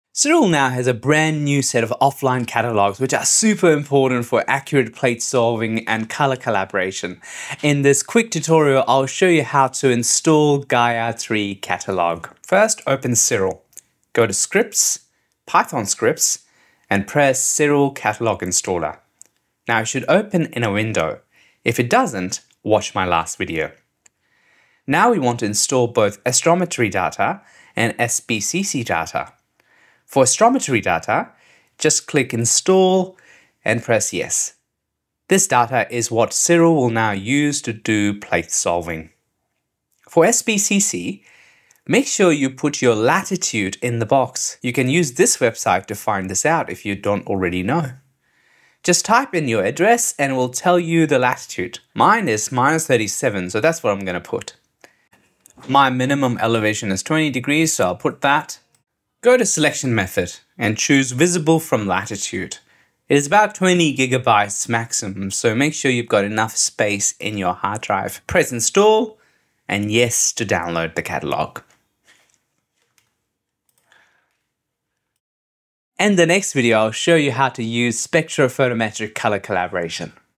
How to Install Powerful Gaia DR3 Catalog in Siril 1.4 - Beginners Tutorial-
In this quick video, I will show you how to install powerful Gaia DR3 Catalog in Siril for astrophotography.